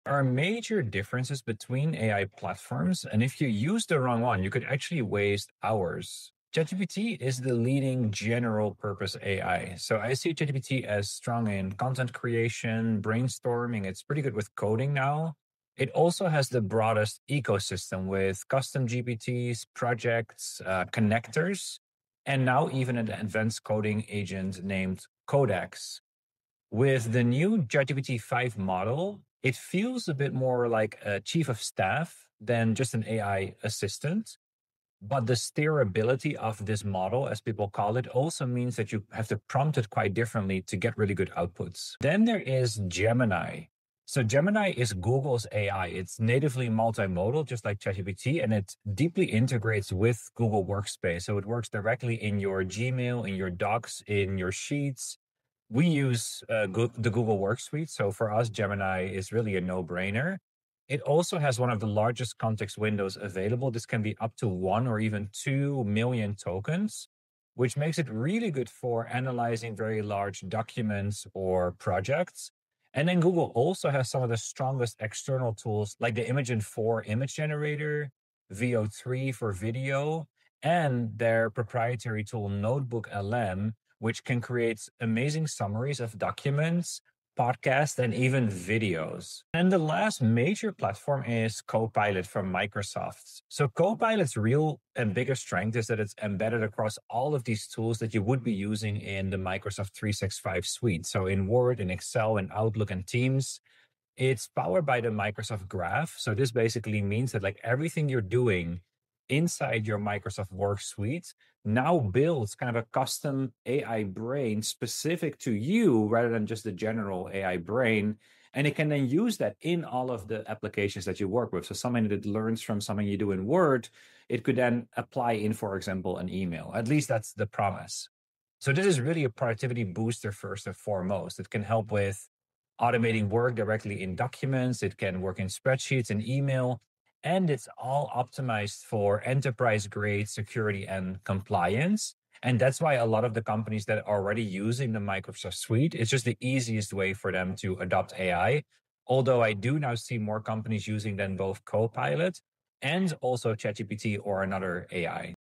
The Executive Guide to AI Platforms: What Leaders Need to Know
AI platforms all look powerful. The real question for leaders is: which one actually gives your team an edge?